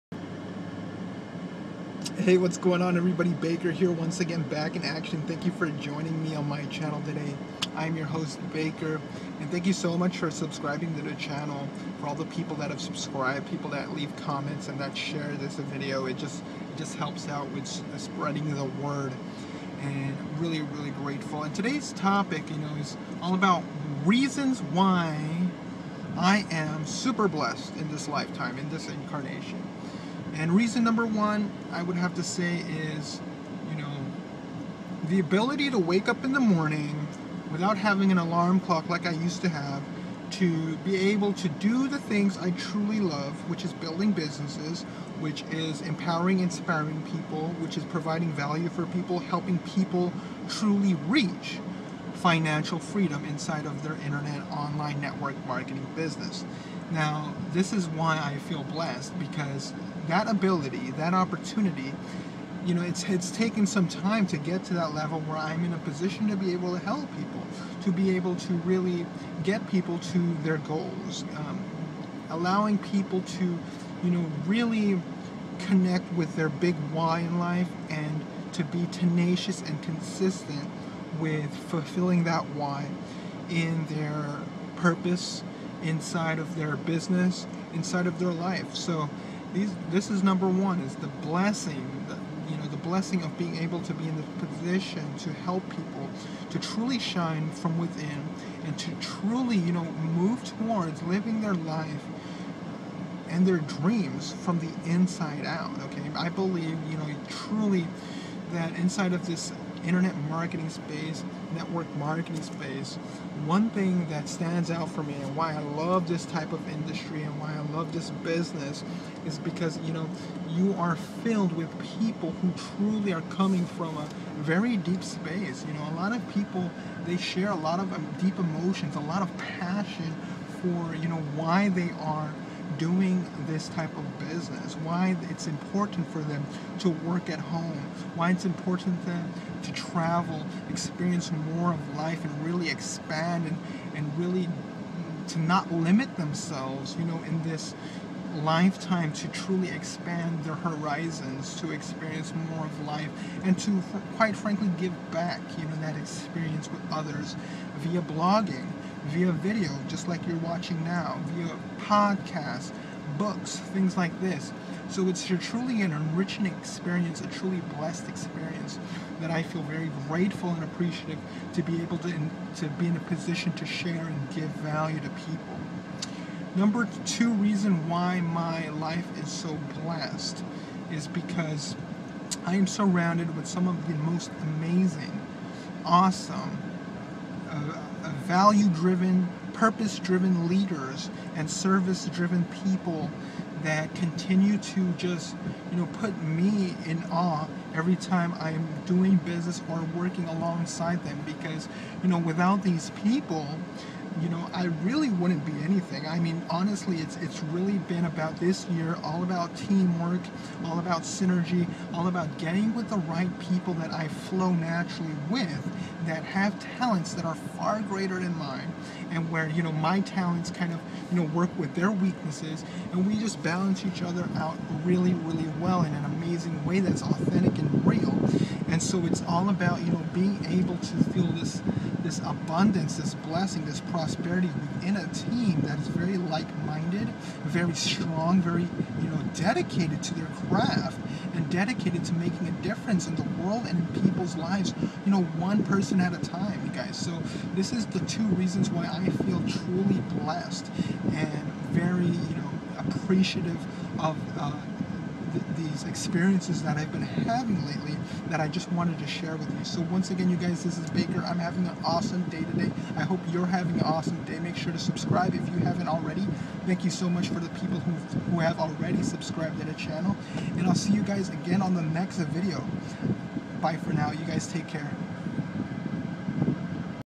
Success tips network marketing
Success Tips for Network Marketing.
Here I share with you successful tips for your network marketing business.

Like my Online Marketing FB page where I share secret online marketing tricks that will help you with your network marking business.